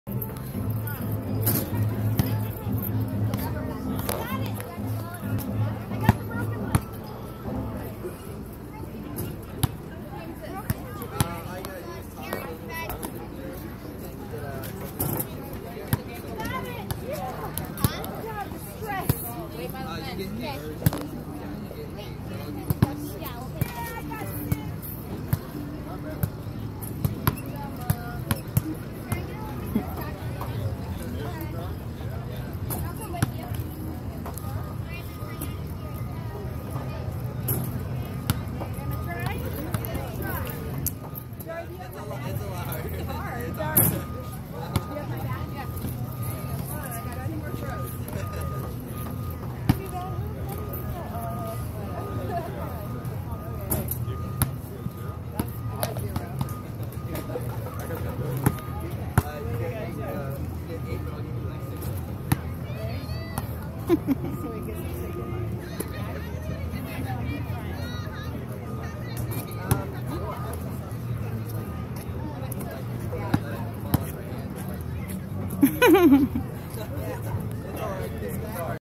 Playing basketball near the Football field...
I went 9/11 from the field and won 5 pirate tokens!